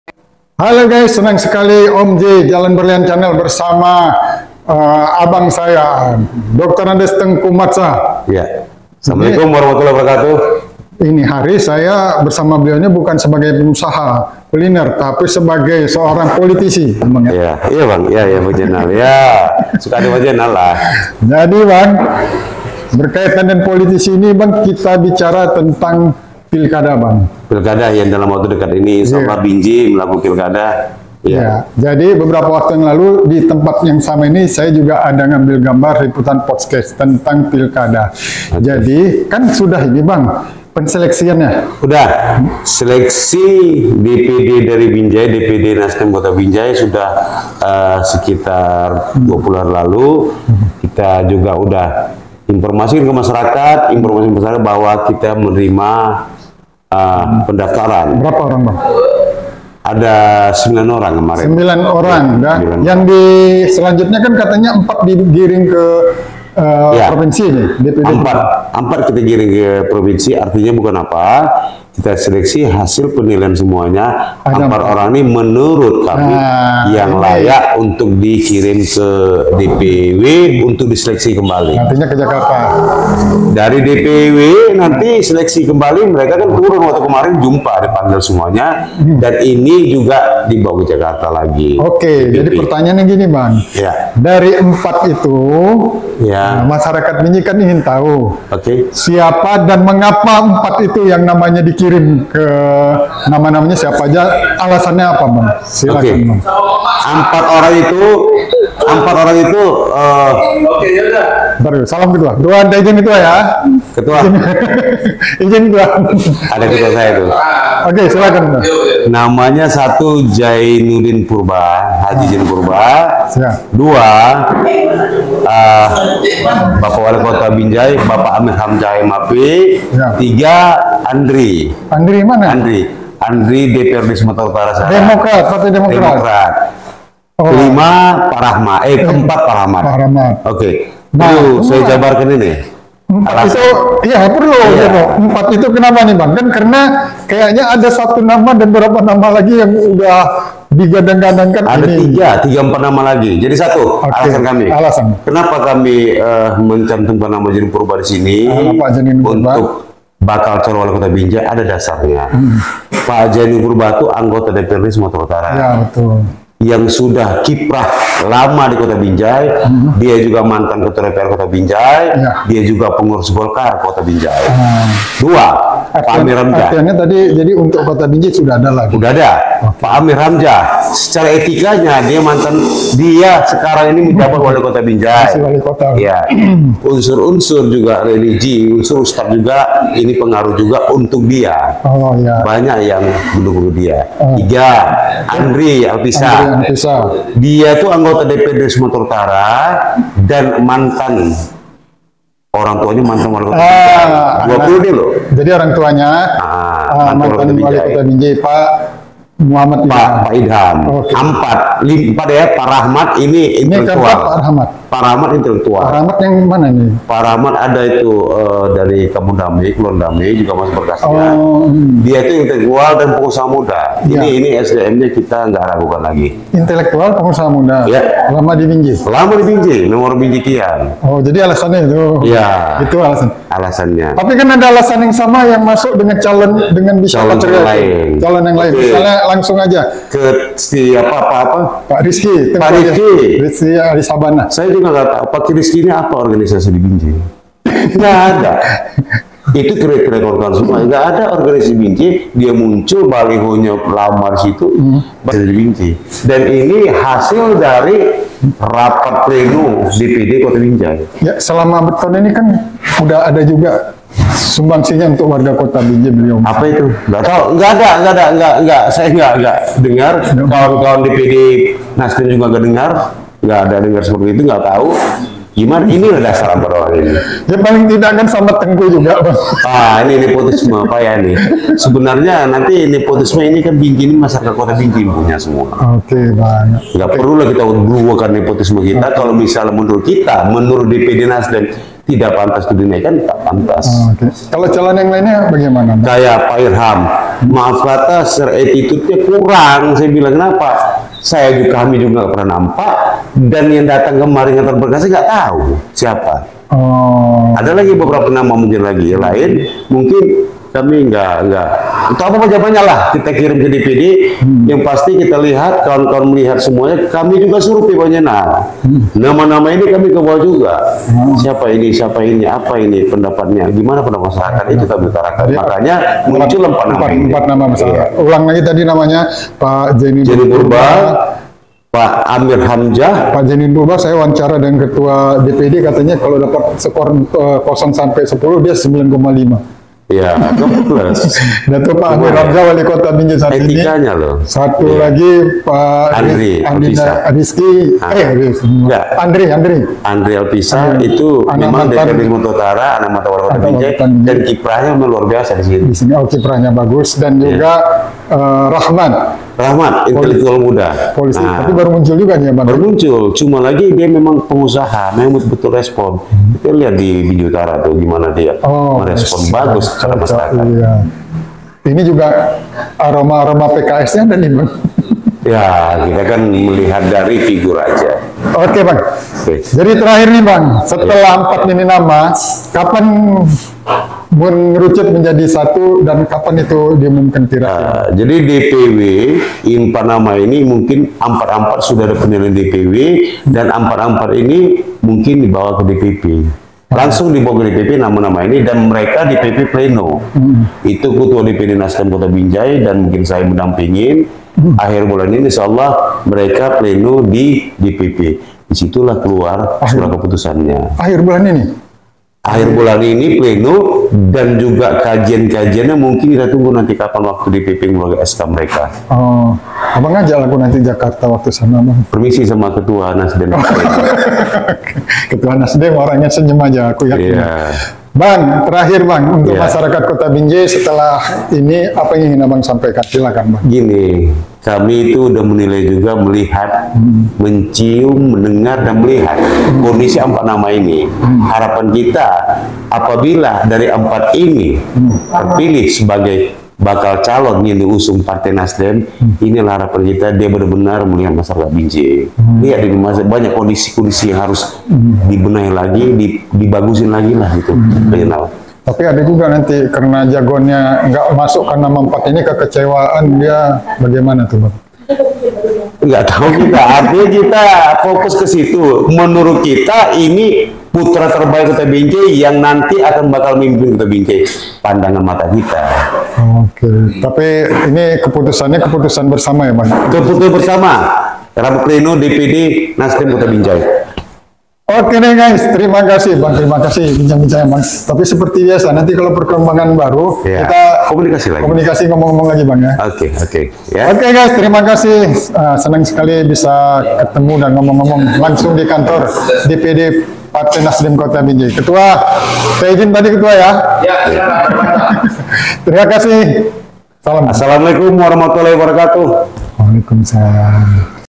RIZKY TAK MASUK 4 BESAR NASDEM BINJAI. T MATSYAH MEMAPARKAN ALASANNYA.
Partai Nasdem kota Binjai mengirimkan empat nama untuk bakal calon walikota binjai.
Yang menarik, salah seorang tokoh pemuda yg di perkirakan akan mendapatkan restu partai nasdem binjai, ternyata tak masuk dalam kategori untuk menjadi calon walikota binjai dari partai nasdem binjai.